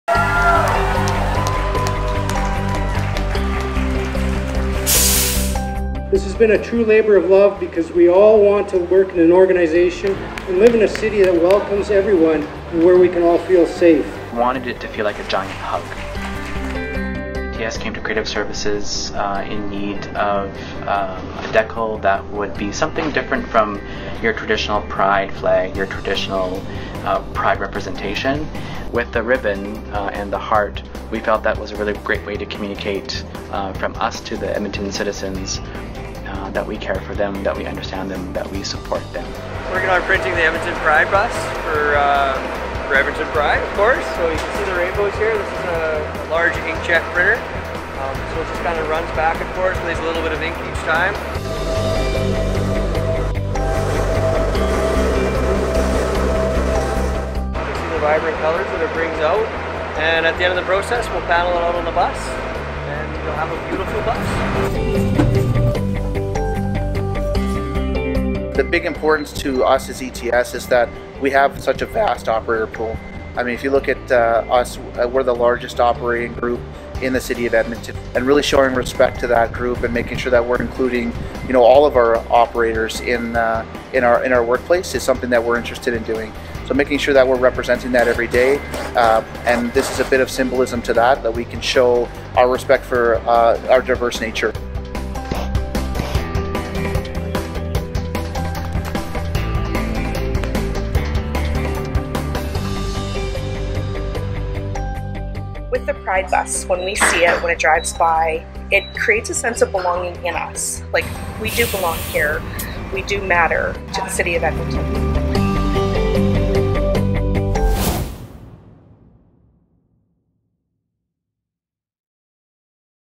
ETS Pride Bus
Here's a look behind that scenes at some of the people and the passion that went into the making of the Edmonton Transit Service (ETS) Pride Bus, unveiled during a ceremony across from City Hall to mark Pride Week 2019. Welcome aboard!